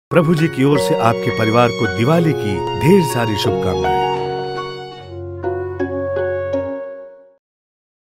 Prabhuji Soanpapdi
This Diwali bring home a pack of sweetness from Prabhuji and celebrate the festival of lights and joy with your loved ones.